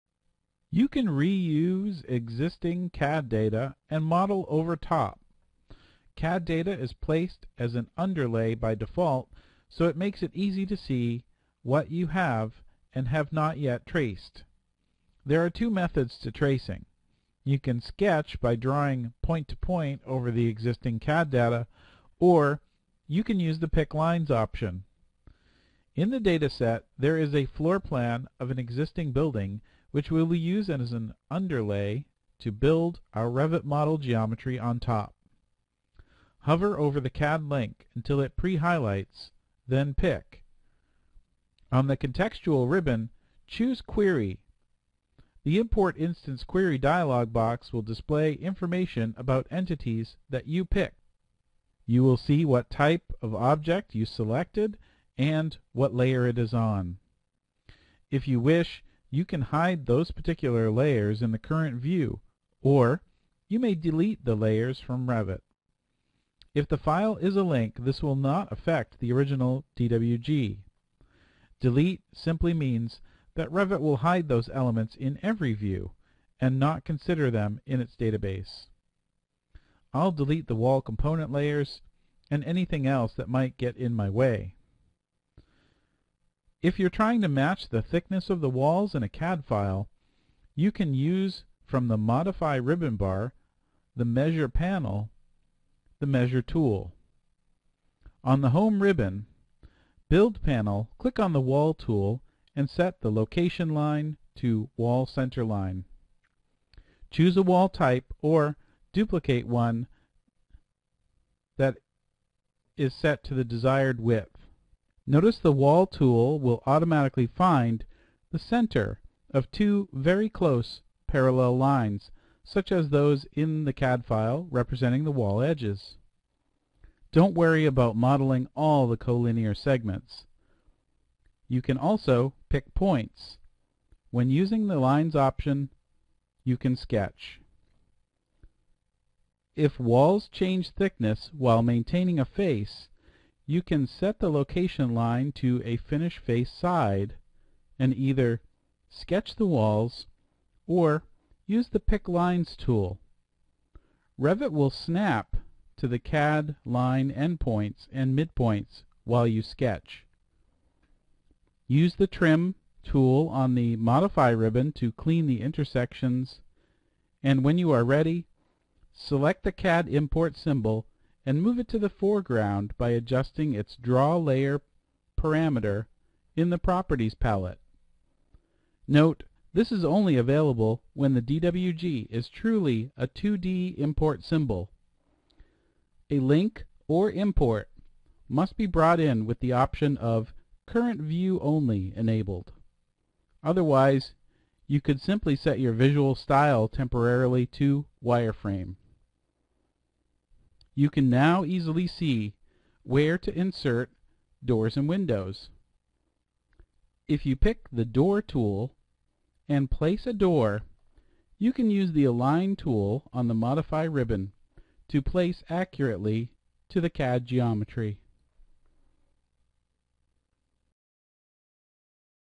Revit Architecture 2011 Tutorial - Using Imported CAD Files to Create Building Model Objects
Reusing existing CAD data as an overlay or underlay on existing models